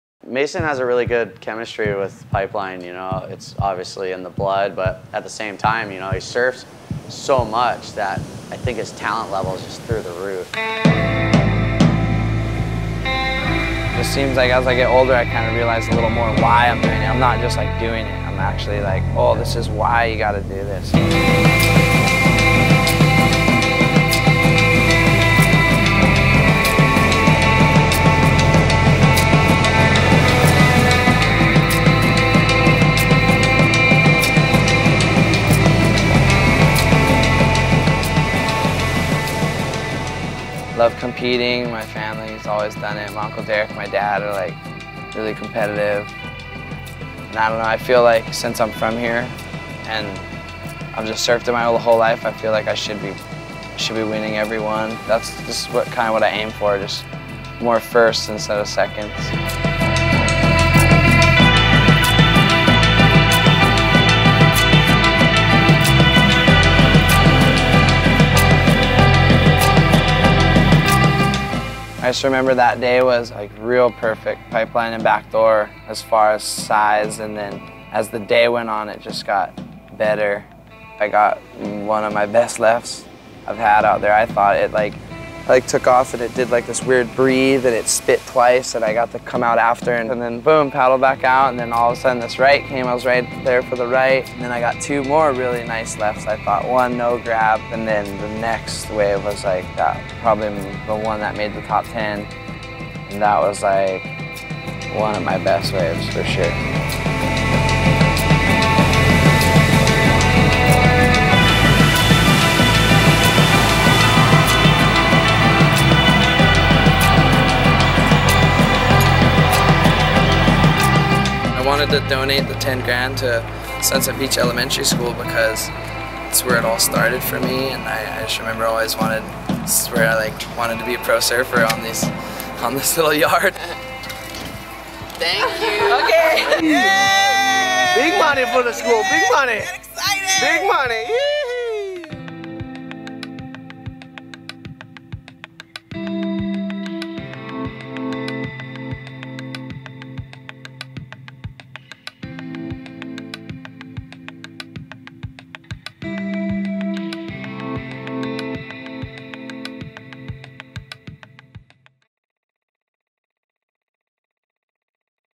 Mason Ho's Groundbreaking Winter of 2014/15
Mason Ho was deemed the winner of the Clif Bar Overall Performance Award by the Wave of the Winter Judging Panel of Gerry Lopez, Pancho Sullivan, Ross Williams and Shawn Briley. He received $10,000 and also a $10,000 donation to the charity of his choice. Ho selected Sunset Beach Elementary School, since that was where his affinity for Pipeline was born.

The OPA is given to the surfer who demonstrates, more than any other, outstanding skill, commitment to both the sport and community, courage and style while consistently riding the most challenging waves on the North Shore, during the entire WOTW contest period.